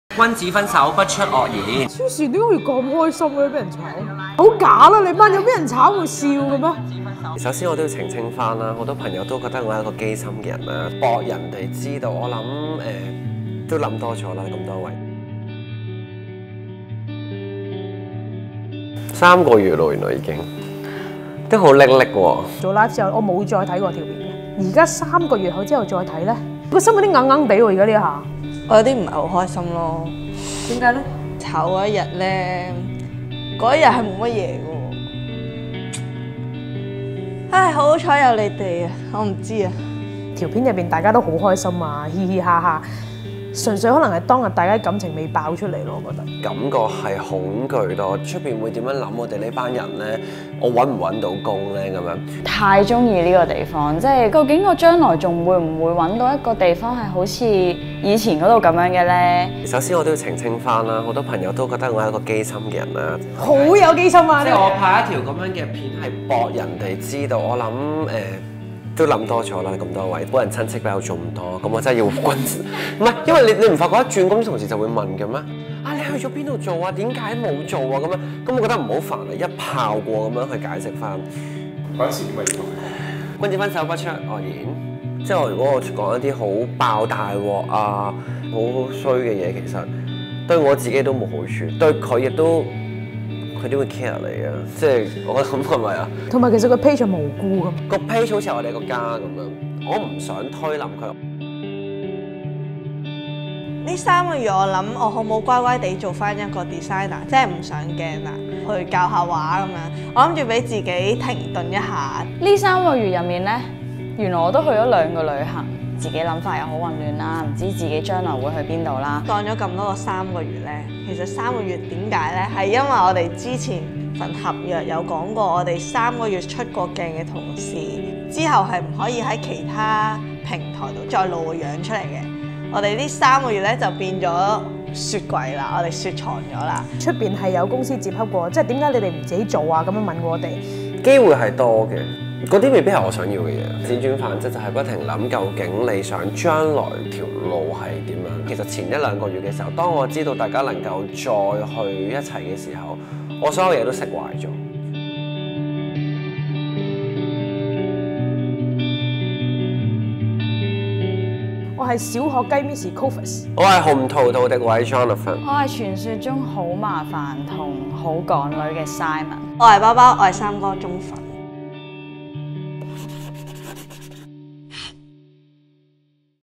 The Mass Laid Off STAKKers
In the past one year, they have been working for STAKK, a top digital media in Hong Kong. They produced over a hundred of viral videos and attracted over 700,000 fans. However, due to a sudden decision made by the CEO, 30 staffs were fired in a day without advance notice. After "disappearing" from the internet for 3 months due to contract issue, they are now here responding to the incident... ...